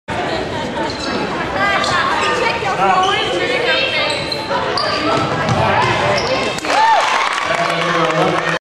Excellent Sideline OOB
The sophomore Hawks run their sideline play perfectly.